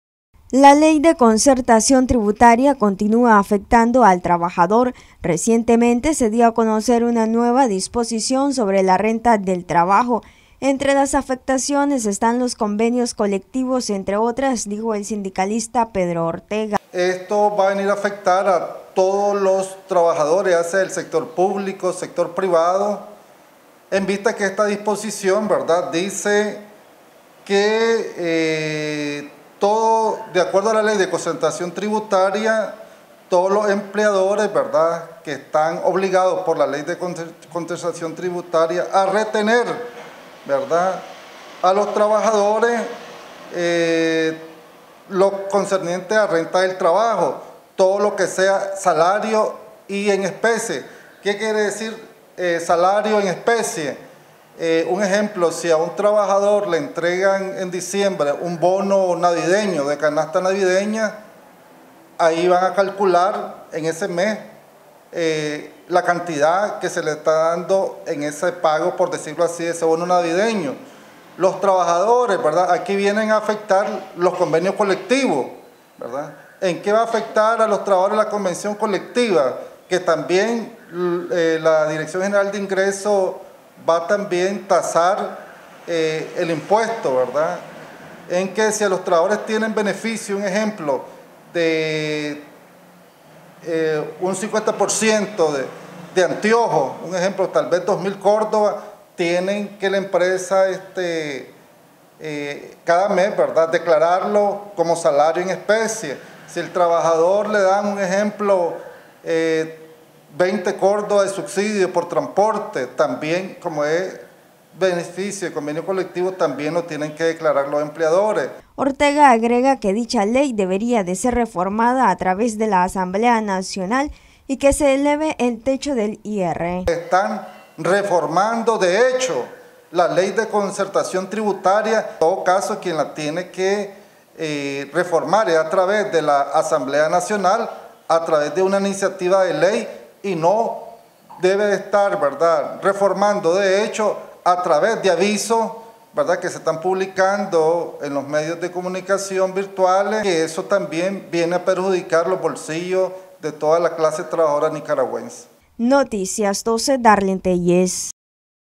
Ley de Concertación Tributaria afecta a la clase trabajadora, según Sindicalista
Noticias12Nicaragua Es necesario que la Ley de Concertación Tributaria sea reformada ya que continúa afectando al trabajador, tomando en cuenta las nuevas disposiciones sobre la Renta del Trabajo, dijo el sindicalista Pedro Ortega.